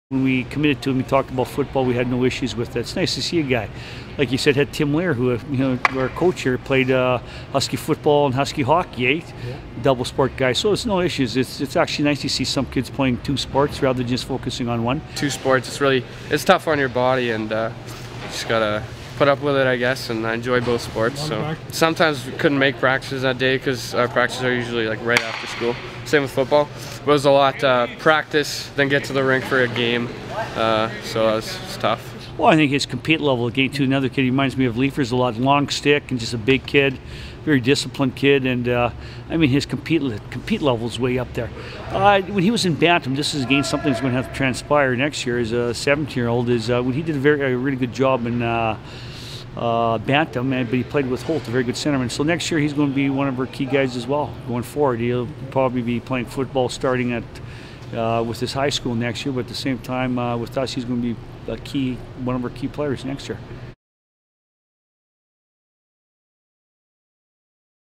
Saskatoon Contacts huddle up around QB Jonah Bumphrey, a cool customer on ice and gridiron
The 16-year-old Bumphrey is a two-sport athlete still pursuing football as a quarterback and forward in hockey. Bumphrey and the Saskatoon Contacts are headed to New Brunswick for the 2016 Telus Cup national midget hockey championship.